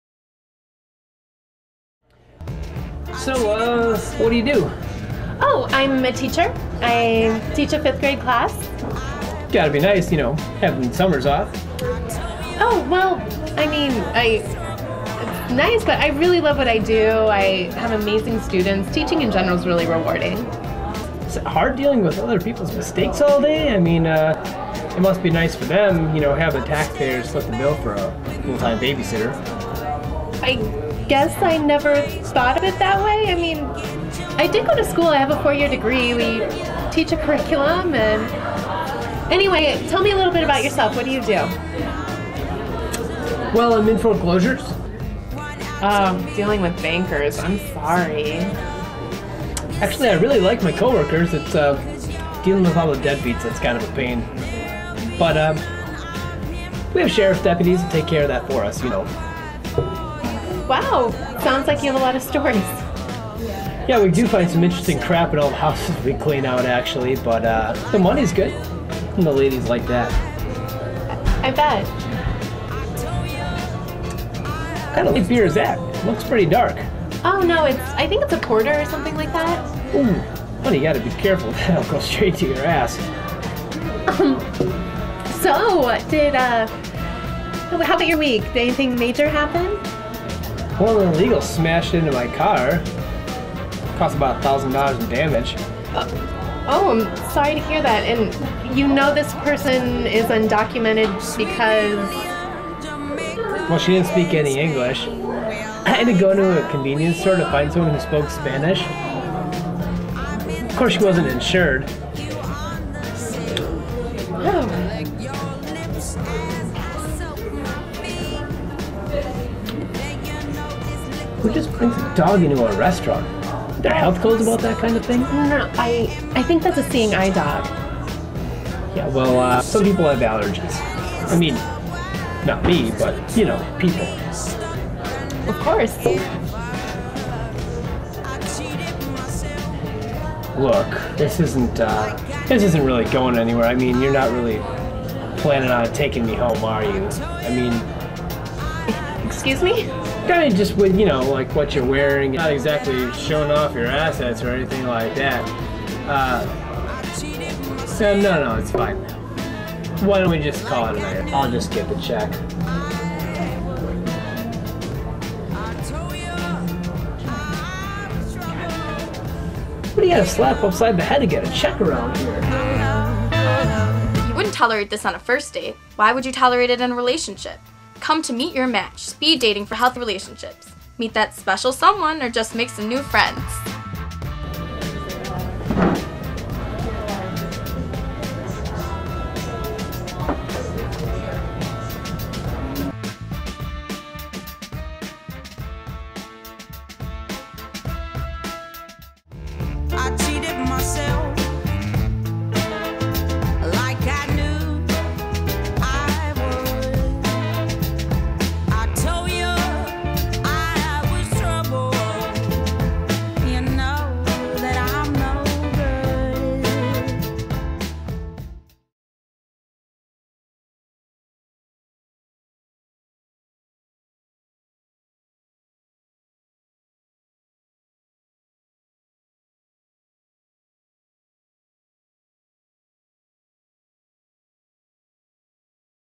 Meet Your Match: Speed-Dating for Healthy Relationships
Want to help out your community? Looking for love or friends in Madison? Speed-date your way to a healthy relationship while raising money for an excellent cause!

A Fundraiser for Domestic Abuse Intervention Services (DAIS)

Thursday, April 2nd, 2009
7 p.m.-9 p.m. (Doors open at 6 p.m.)
The Brink Lounge
701 E. Washington Ave., Madison, WI

-Participate in either Speed-dating or Speed-friending
-Meet a new flame or friend!
-Open to daters of all orientations / LGBT daters welcome
-Drinks, free appetizers, and amazing raffle prizes!
- Free admission to the after party
- Hosted by NBC15's Leigh Mills!

Admission: $25
Help cover the cost and raise more money for DAIS by getting pledges from your family and friends!*

*Receive 1 raffle ticket for every $5 or 3 raffle tickets for every $10 raised above the admission price, and get more chances to win one of our many great prizes!

~Meet your Match is organized by an independent local group of graduate students and young professionals to benefit DAIS~